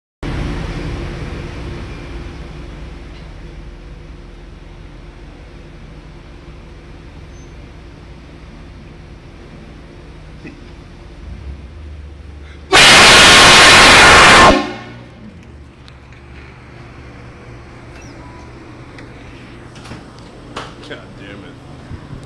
Air horn wake up
Attempting to wake up a vagabond with air horns.